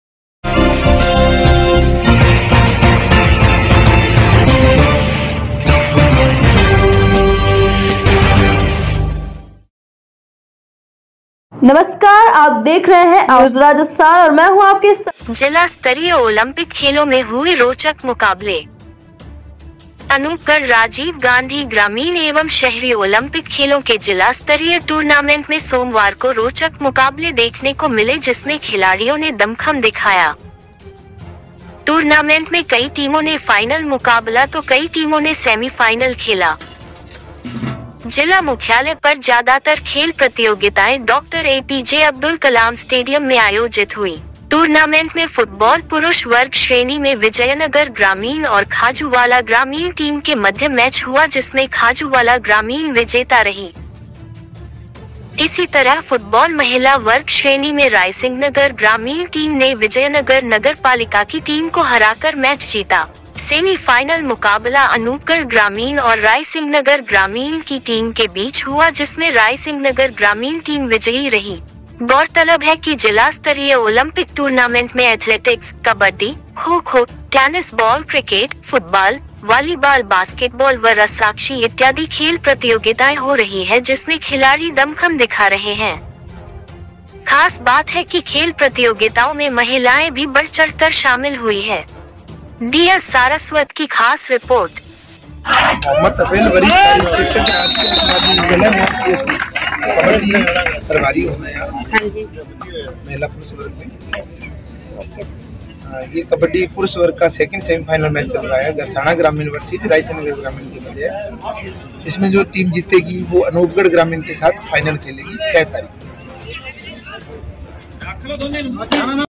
जिला स्तरीय ओलंपिक खेलों में हुए रोचक मुकाबले
ANR news Rajasthan is India,s best Hindi news channel.

 NEWS news channel covers the latest news in politicals, entertainment, Bollywood,,,

*डी एल सारस्वत अनूपगढ की रिपोर्ट*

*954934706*

अनूपगढ़, 04 सितंबर। राजीव गांधी ग्रामीण एवं शहरी ओलंपिक खेलों के जिला स्तरीय टूर्नामेंट में सोमवार को रोचक मुकाबले देखने को मिले जिसमें खिलाड़ियों ने दमखम दिखाया। टूर्नामेंट में कई टीमों ने फाइनल मुकाबला तो कई टीमो ने सेमीफाइनल खेला। जिला मुख्यालय पर ज्यादातर खेल प्रतियोगिताएं डॉ एपीजे अब्दुल कलाम स्टेडियम में आयोजित हुई। टूर्नामेंट में फुटबॉल पुरुष वर्ग श्रेणी में विजयनगर ग्रामीण और खाजूवाला ग्रामीण टीम के मध्य मैच हुआ जिसमें खाजूवाला ग्रामीण विजेता रही। इसी तरह फुटबॉल महिला वर्ग श्रेणी में रायसिंहनगर ग्रामीण टीम ने विजयनगर नगर पालिका की टीम को हराकर मैच जीता। सेमी फाइनल मुकाबला अनूपगढ़ ग्रामीण और रायसिंहनगर ग्रामीण की टीम के बीच हुआ जिसमें रायसिंहनगर ग्रामीण टीम विजयी रही। गौरतलब है कि जिला स्तरीय ओलंपिक टूर्नामेंट में एथलेटिक्स, कबड्डी, खो-खो, टेनिस बॉल क्रिकेट, फुटबाल, वालीबाल बास्केटबाल व रस्साकशी इत्यादि खेल प्रतियोगिताएं हो रही है जिसमें खिलाड़ी दमखम दिखा रहे हैं। खास बात है कि खेल प्रतियोगिताओं में महिलाएं भी बढ़ चढ़कर शामिल हुई है।